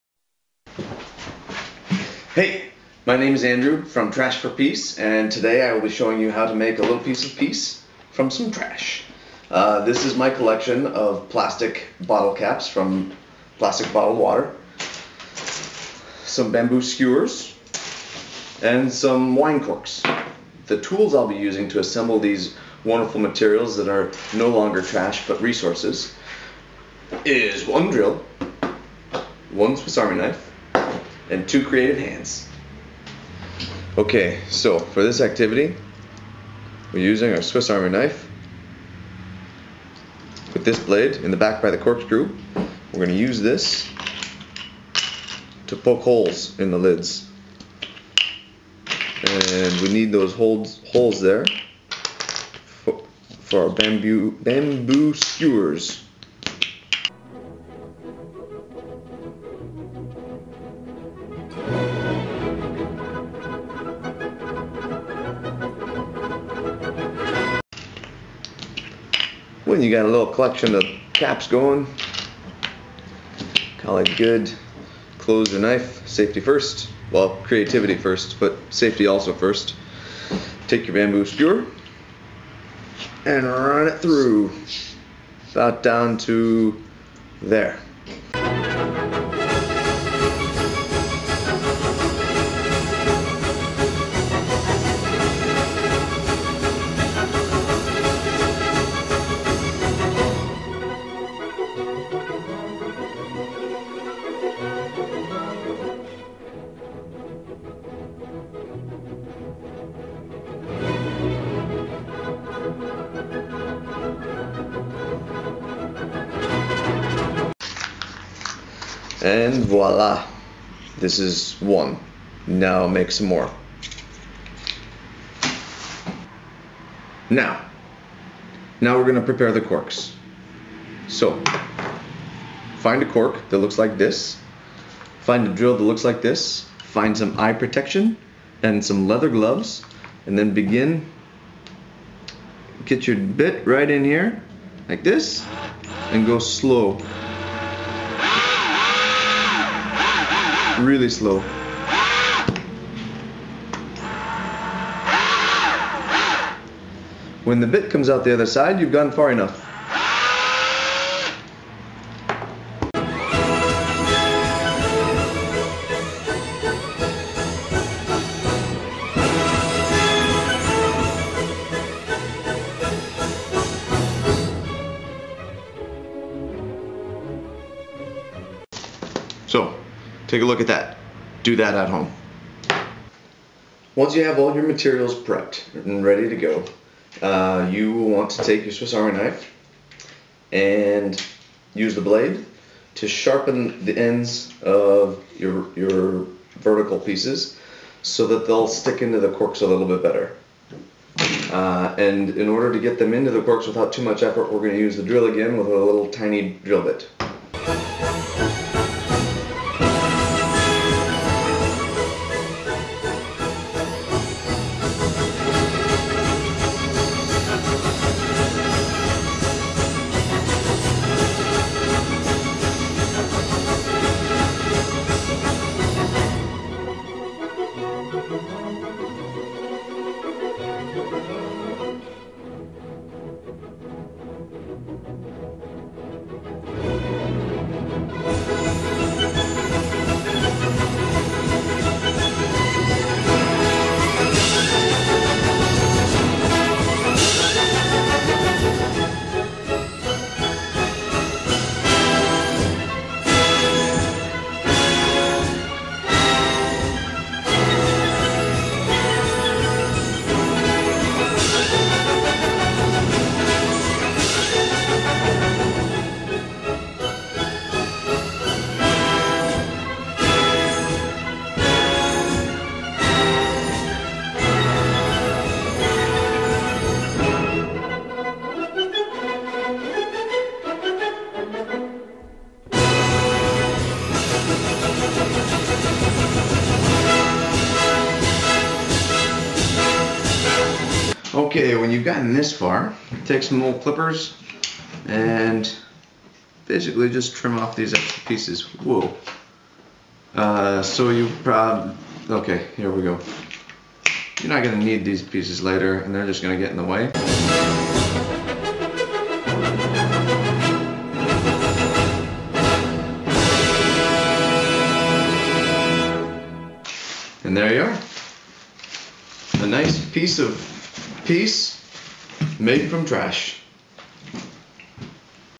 Bottle Cap Bin!
Willy nilly might get you lucky, but having a plan will bring you success! Make a plan to be creative, and wrap your mind around the idea of waste as a resource! This video is a super fine example of waste materials that were collected by local bars and restaurants and donated to an innovative cause.

Be safe when you're being creative, consult OSHA's safety standards before you begin a project.

If you know anything about recycling, then you know that those plastic bottle caps are 100% unrecyclable. But, if you know anything about innovation, then you know that they are 100% reusable!